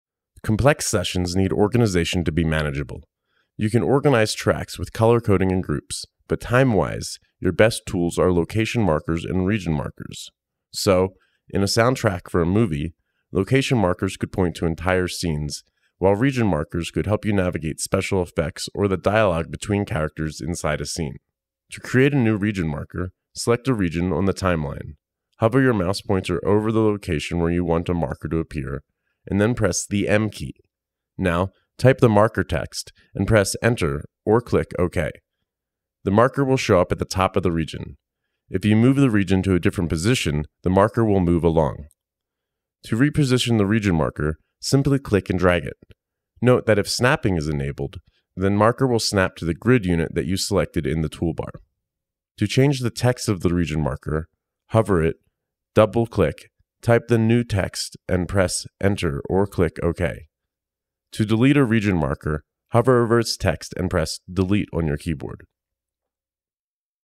Region markers in Ardour
Complex sessions need organization to be manageable. You can organize tracks with color-coding and groups, but time-wise, your best tools are location markers and region markers. Here is how you can create and edit the latter.